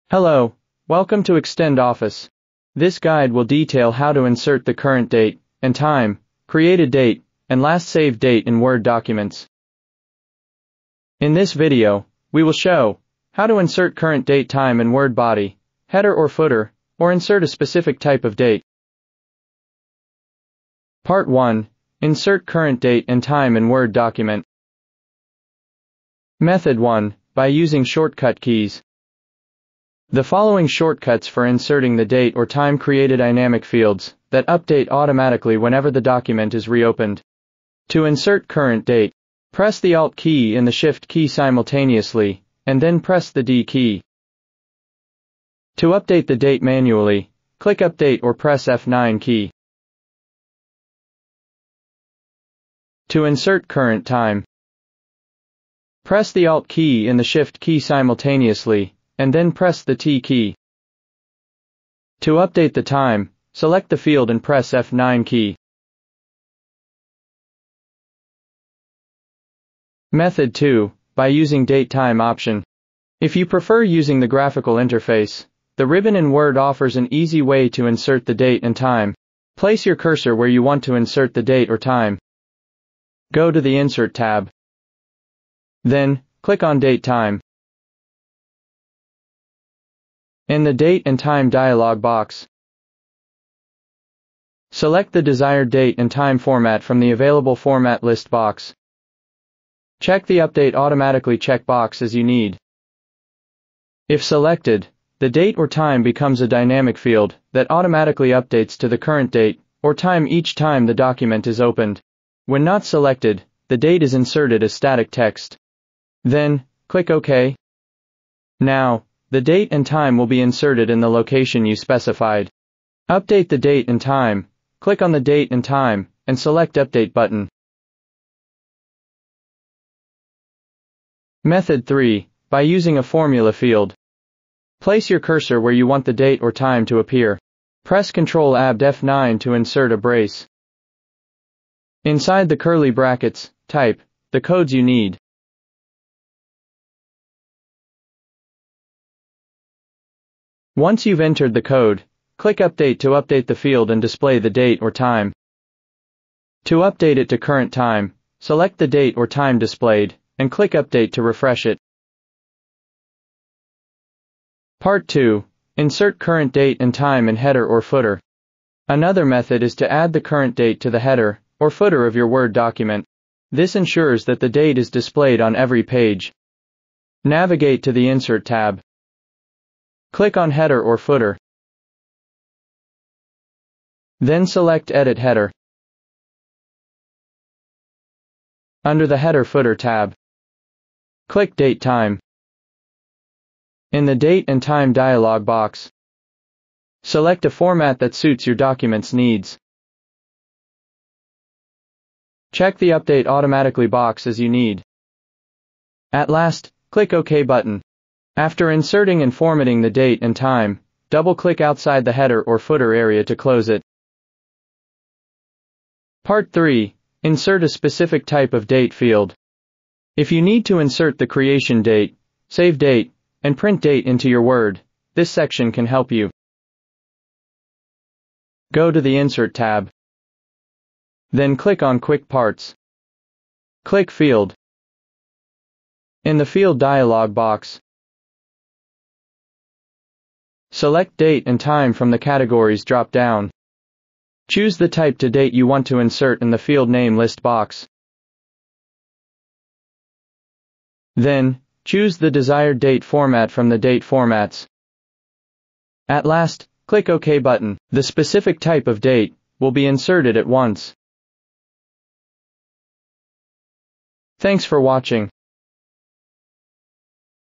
Quickly insert current date and time in Word – (Easy tricks)
When creating and managing documents in Microsoft Word, the ability to insert the current date and time is particularly important. This feature not only ensures the accuracy of the records and the authenticity of the documents but also, with an auto-updating timestamp, reflects the most current information every time the document is opened or printed. This video will detail how to insert the current date and time, created date, and last saved date… in Word documents.

00:00 Start
00:21 Part 1: Insert current date and time in Word document
02:45  Part 2: Insert current date and time in header or footer
03:36  Part 3: Insert a specific type of date field (Create date, Save date, Print date)